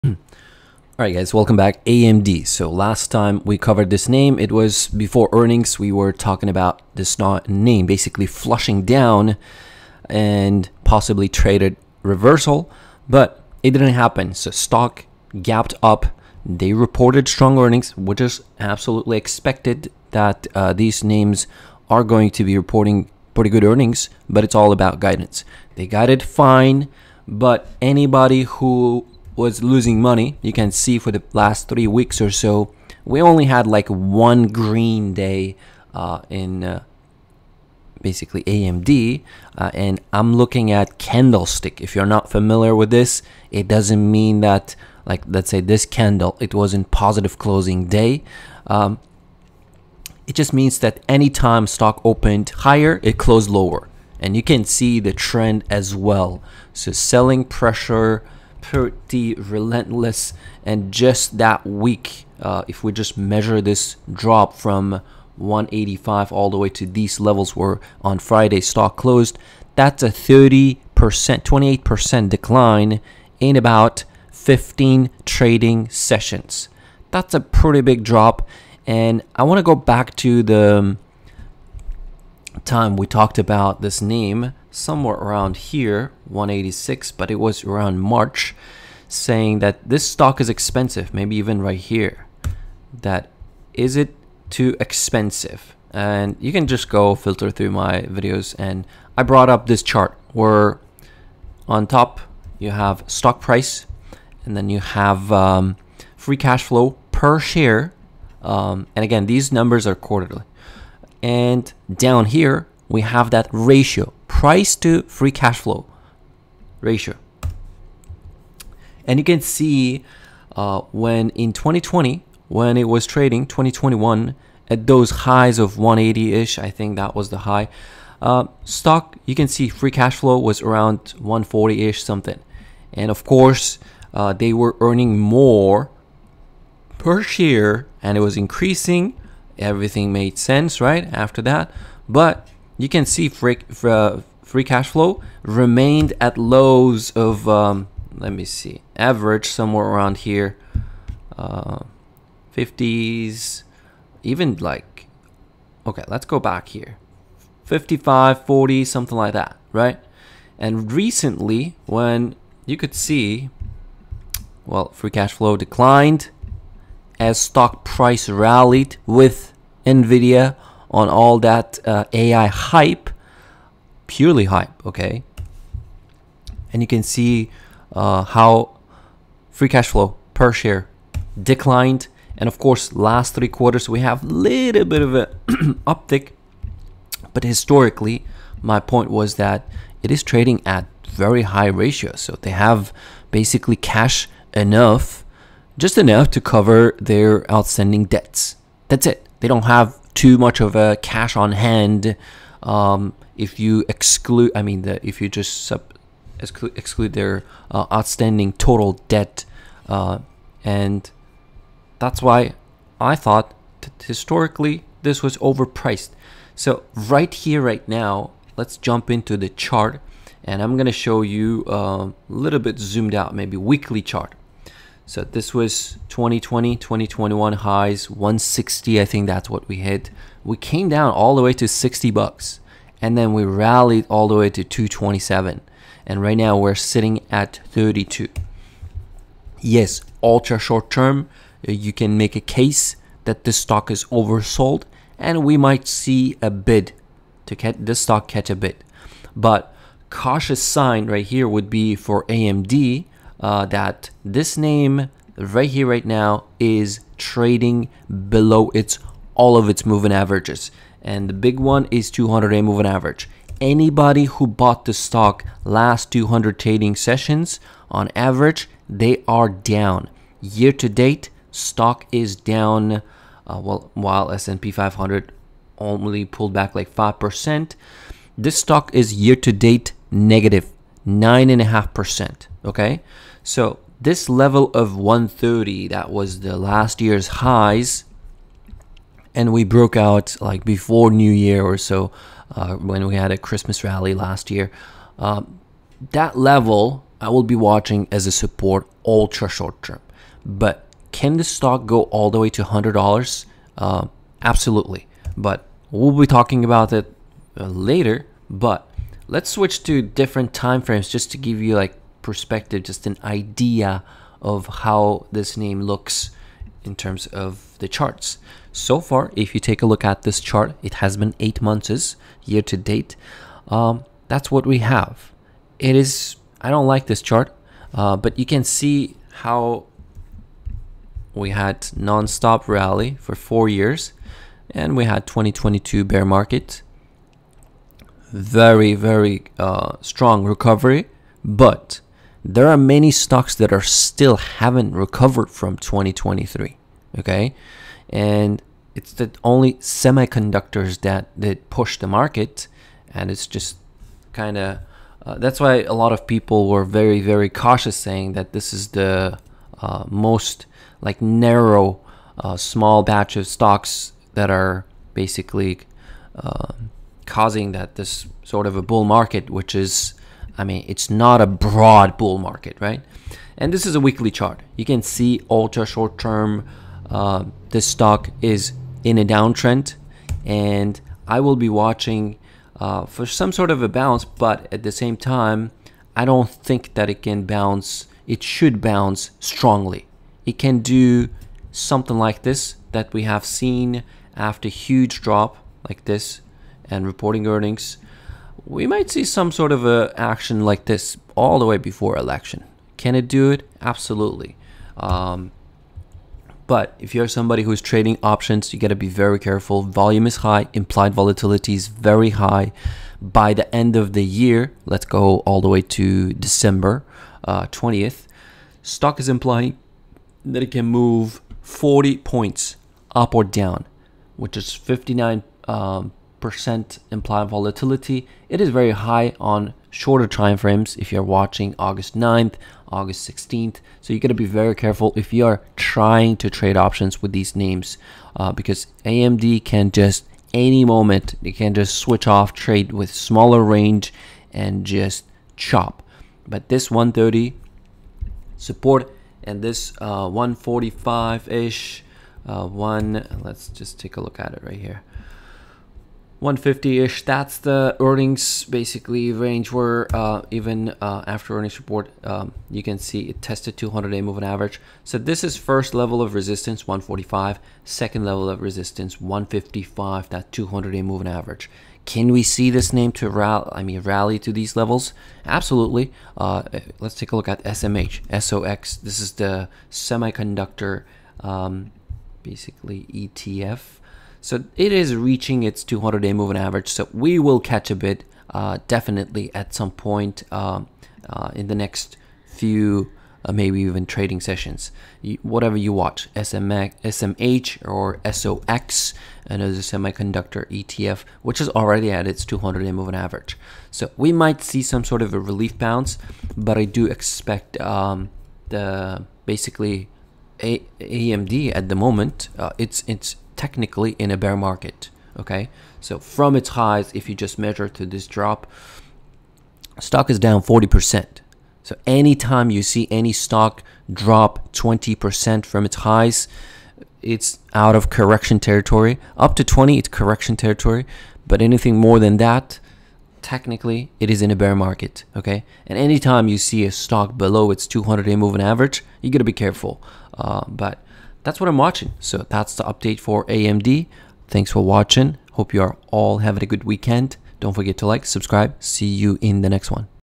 AMD Stock Analysis, In Bear Market! -40%+
Amd stock technicals analysis price action levels to watch